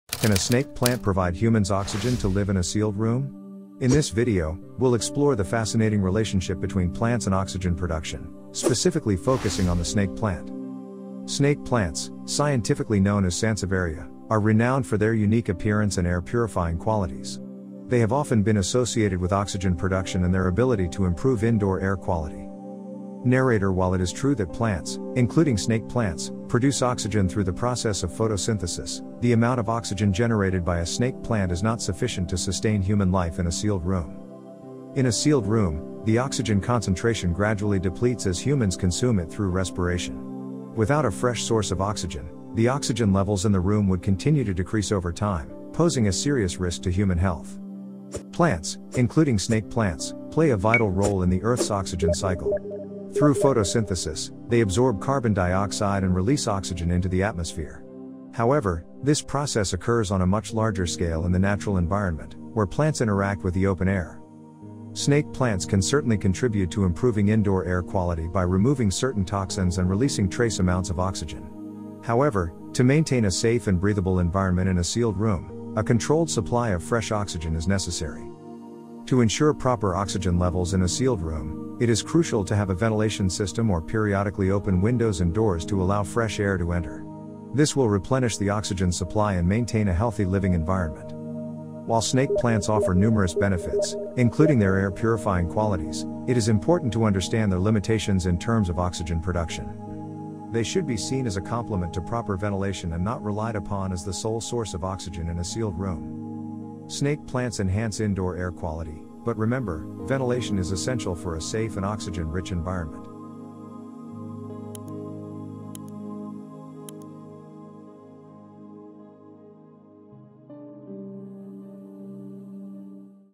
Can a snake plant provide humans oxygen to live in a sealed room? Explainer Video | Leaf Town
Making of:
Copyrights: leaf Town
Director: Ibrahim Kholil
Creator: Monira Alam
Video Editor: Mainu
Band: creator squad